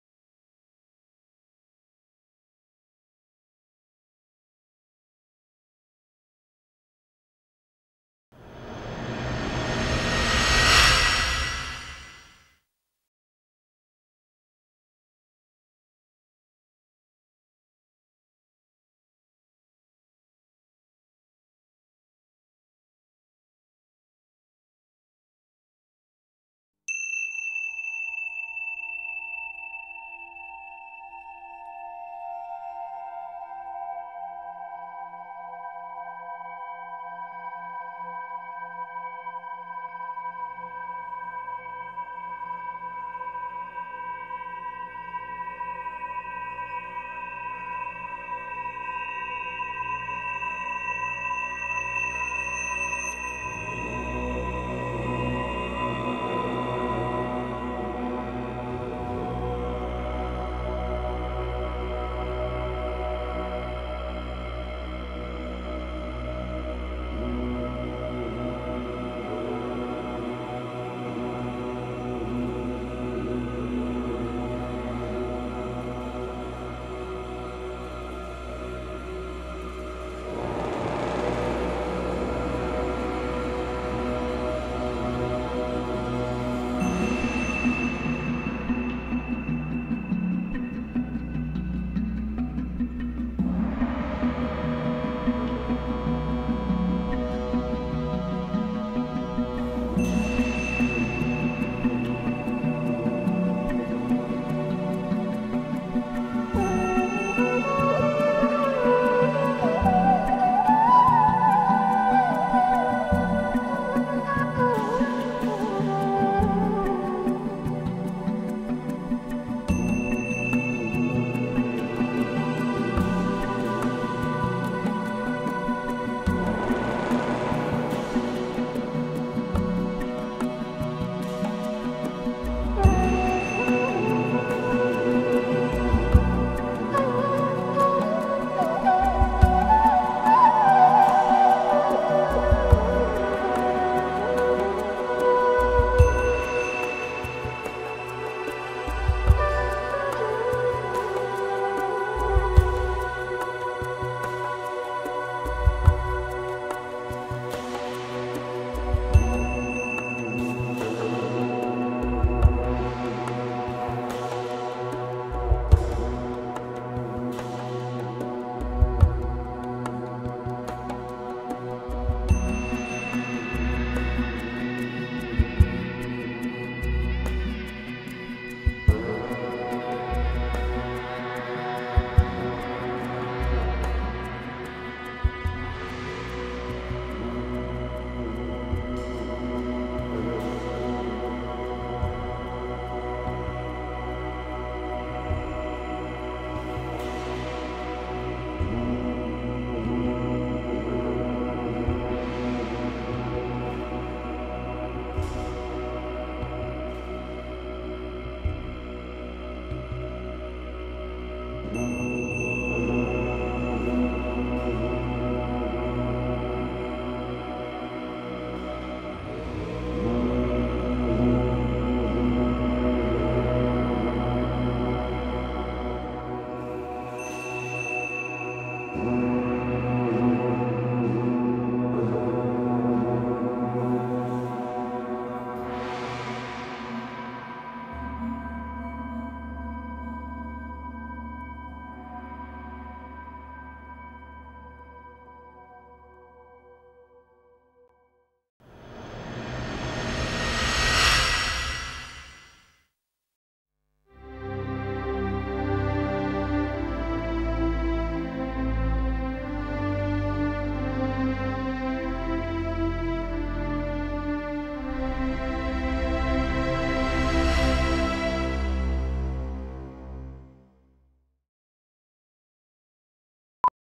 India: The Land of Buddha Celebrating Buddha Purnima (Vesak)
The spirit of Buddhism lies in the simple life that Gautama Buddha embodied and preached. Buddha Purnima, which celebrates the birth, enlightenment and death of the Buddha, is a manifestation of Buddha's invaluable teachings. Celebrated across India on the full moon day of the Vaisakh month of the Hindu calendar, it is a day of solemn celebration wherein devotees gather at the Mahabodhi Temple in Bodh Gaya, Bihar, reflect on their lives and remember the prince who gave up every material possession to gain a light that illuminates the way for all those who follow.